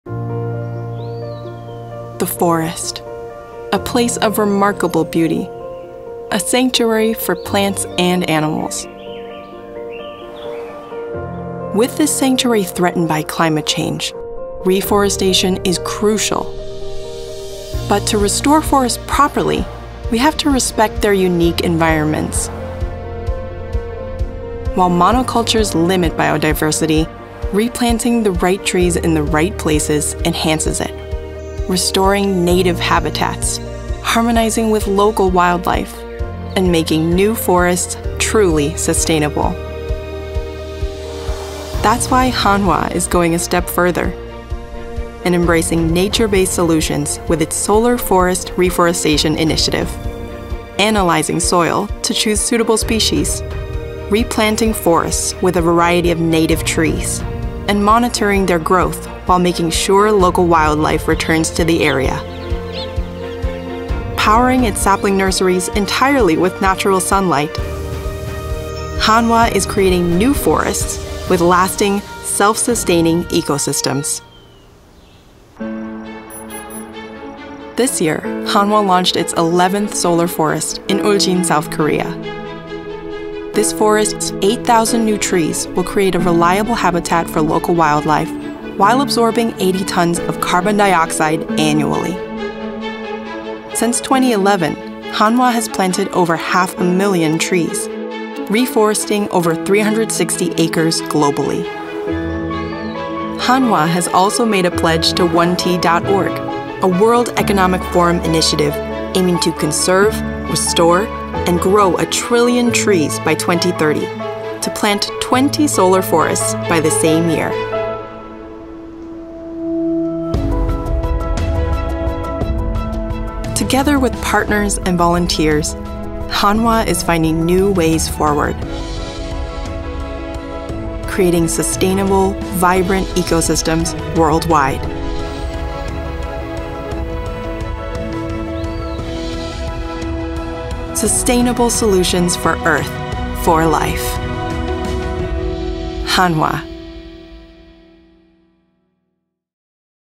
Biodiversity in action: Hanwha's 11th Solar Forest
Forest biodiversity is essential for sustaining life on our planet. Diverse ecosystems purify air and water, regulate climate, and enhance soil fertility, making them crucial to combat climate change. However, habitat destruction, forest fires, and monoculture planting threaten these vital forests. Hanwha's Solar Forest initiative is tackling this issue head-on, especially with its latest project in Uljin, South Korea. By raising native tree saplings using solar panels, they are creating resilient habitats. See how innovative reforestation efforts are paving the way for a greener future.

00:00 Restoring biodiversity
00:42 The Solar Forest Initiative
01:17 Hanwha's 11th Solar Forest
01:43 Hanwha’s commitment
02:01 Creating vibrant ecosystems